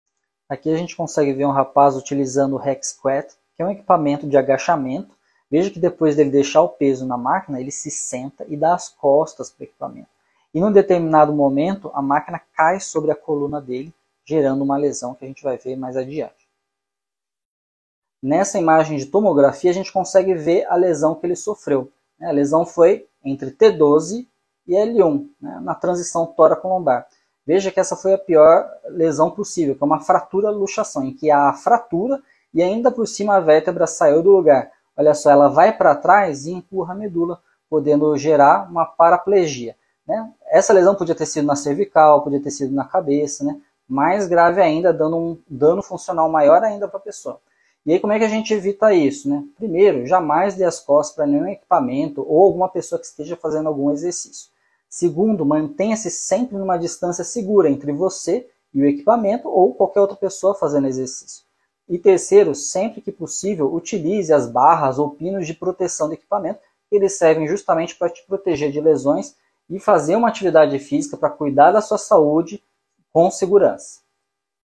Não corra esse tipo de risco na academia!
Aqui mostro um caso de lesão grave na coluna, que pode ser evitada tomando-se alguns cuidados que comento no post.

Dr Carlos Vinícius - CRM 140189